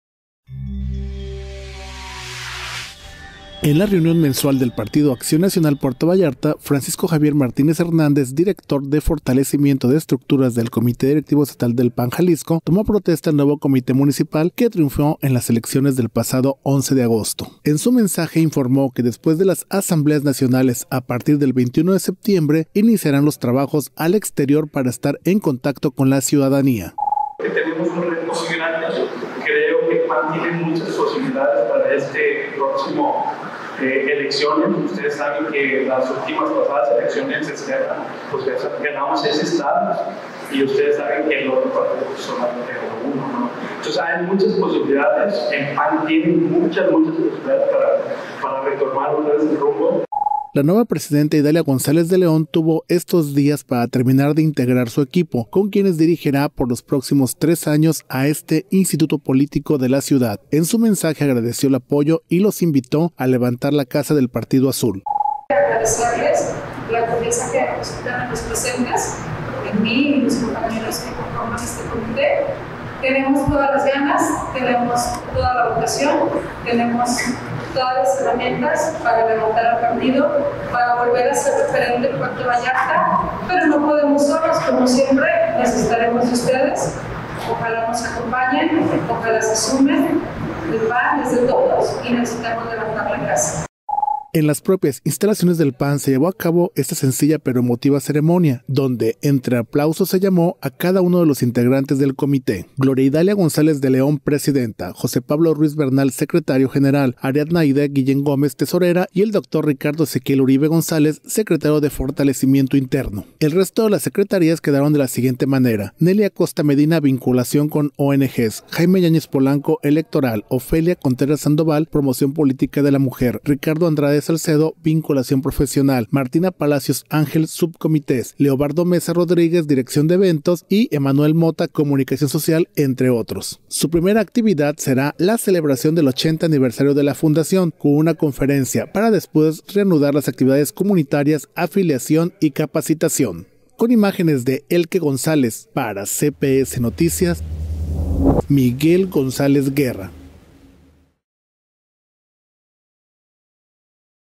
Toma protesta al nuevo CDM del PAN Puerto Vallarta
Puerto Vallarta, Jal.- En la reunión mensual del Partido Acción Nacional Puerto Vallarta, Francisco Javier Martínez Hernández, director de Fortalecimiento de Estructuras CDE, tomó protesta al nuevo Comité Directivo Municipal, que triunfó en las elecciones del pasado 11 de agosto, en su mensaje informó después de las asambleas nacionales, a partir del 21 de septiembre iniciarán los trabajos al exterior, para estar en contacto con la ciudadanía.